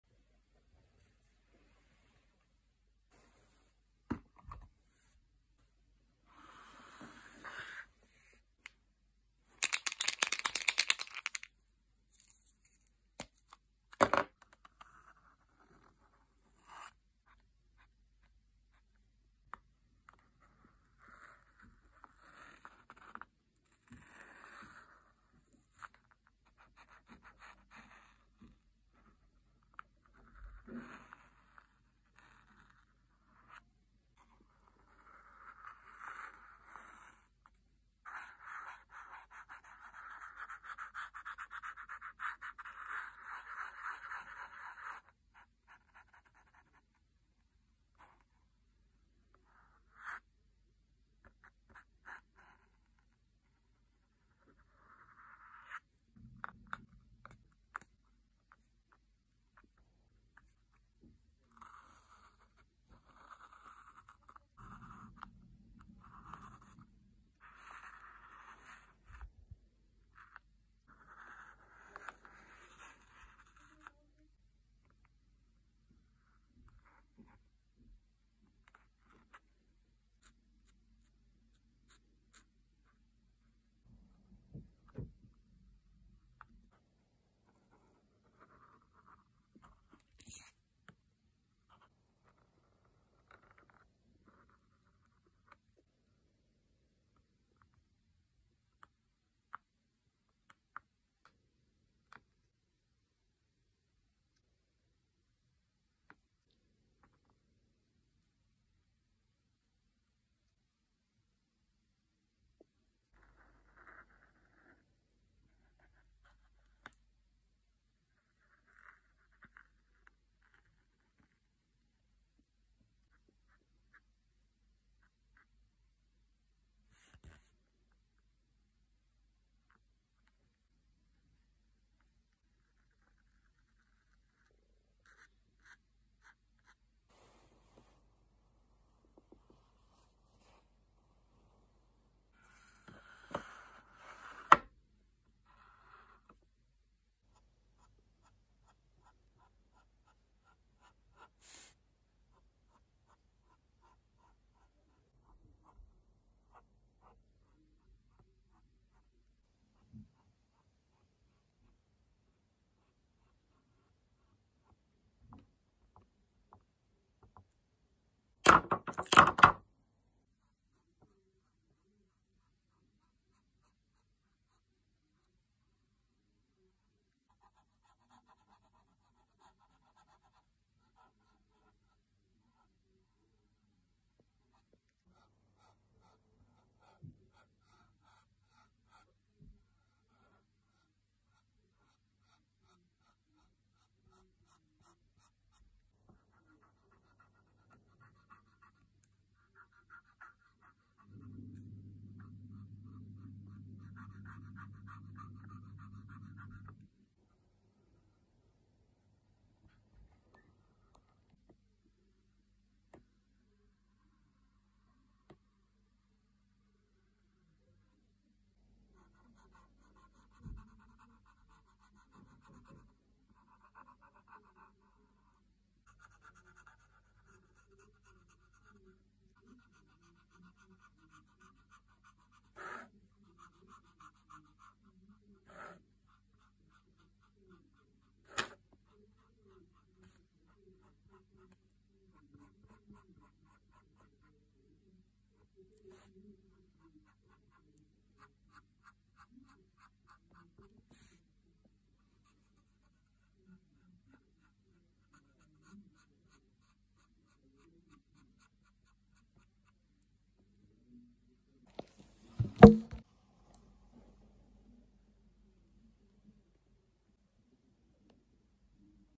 decorate phone cases with me
my friend gave these to me to decorate
I delivered
more will be coming